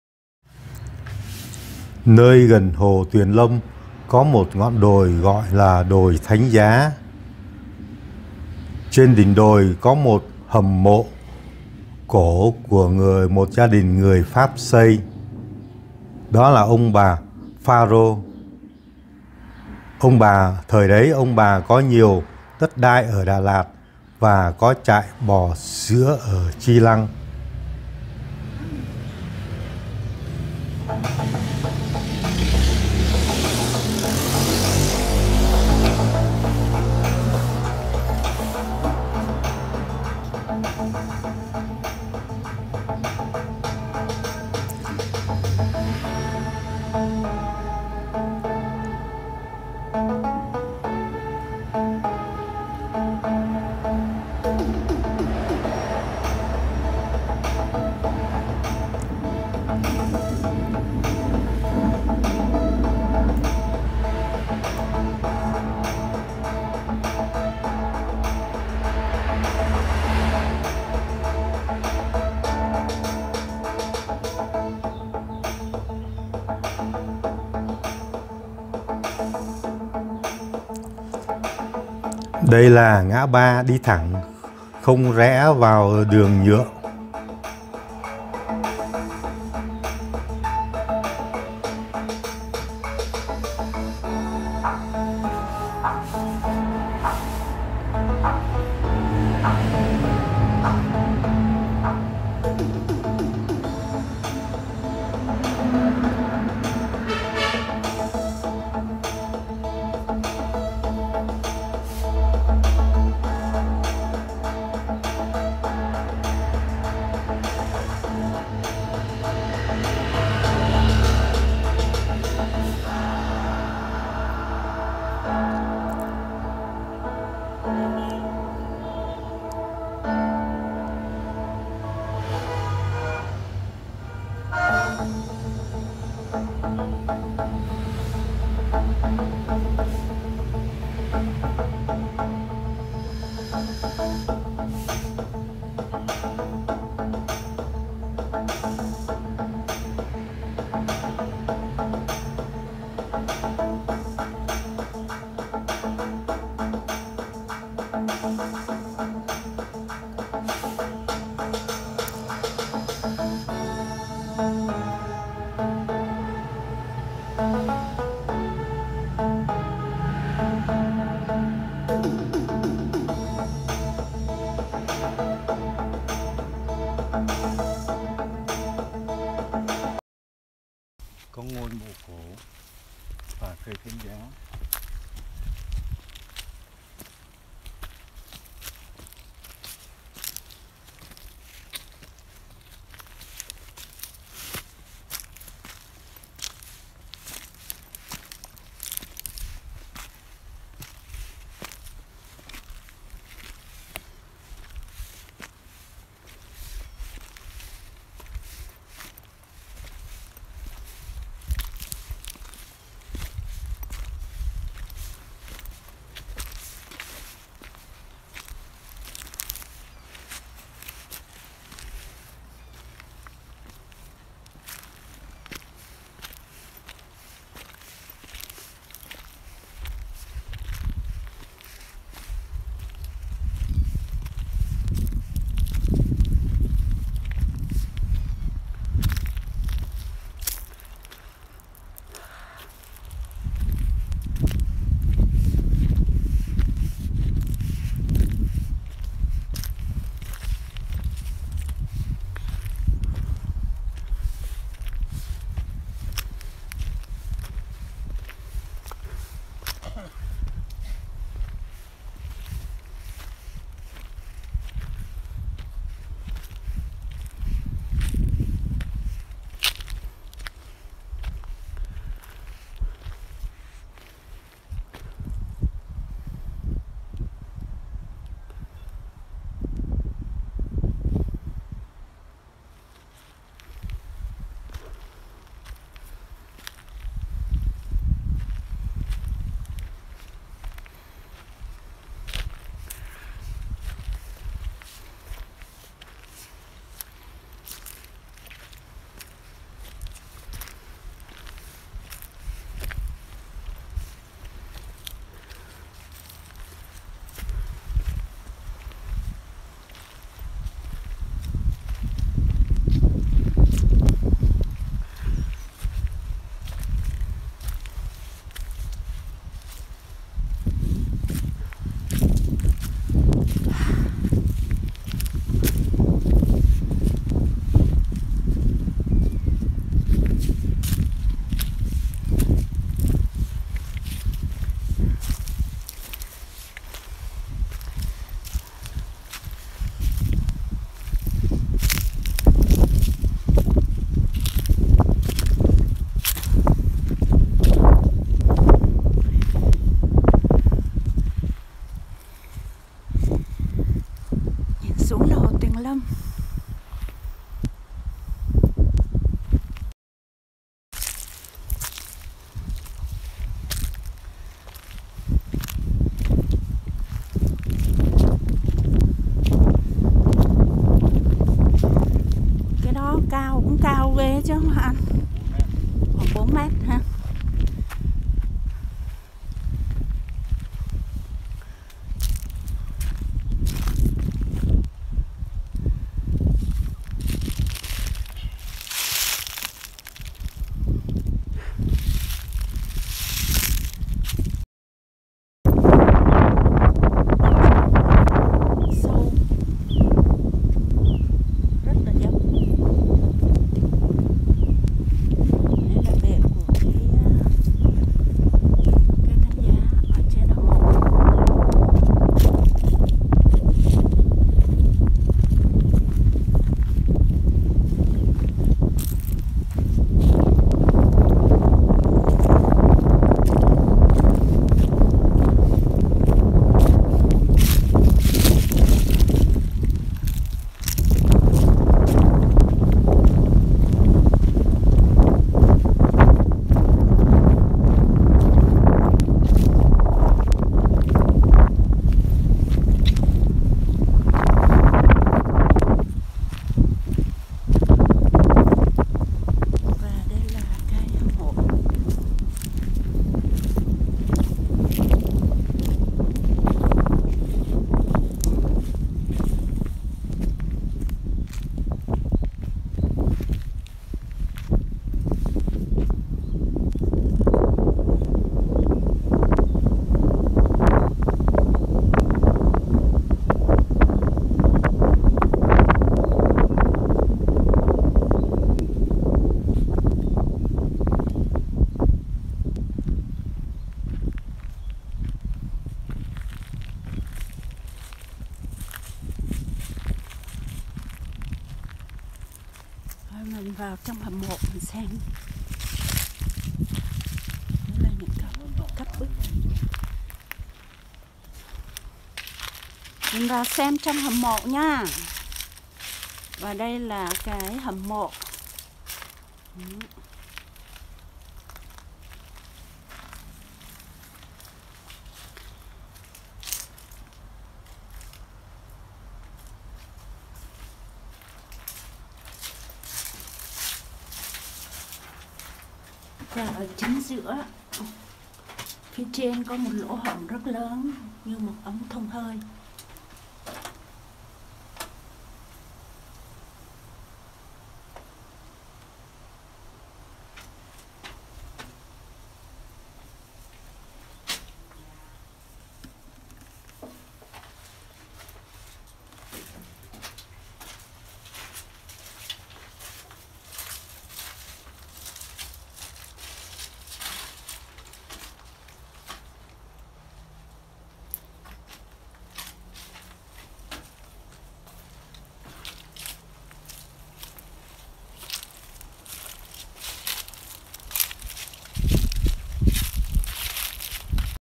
Du lịch Lâm Đồng. Hầm mộ cổ trên đồi Thánh Giá ở Đà Lạt.
Đà Lạt du ký